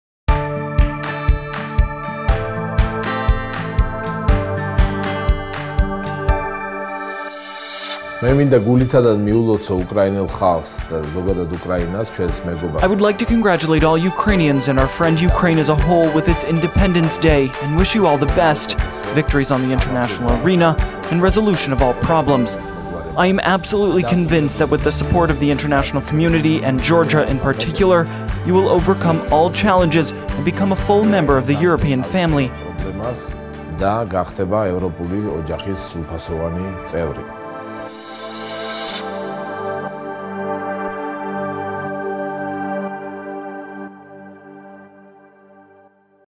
Georgia
Ambassador of Georgia to Ukraine Gela Dumbadze congratulated Ukraine on Independence Day!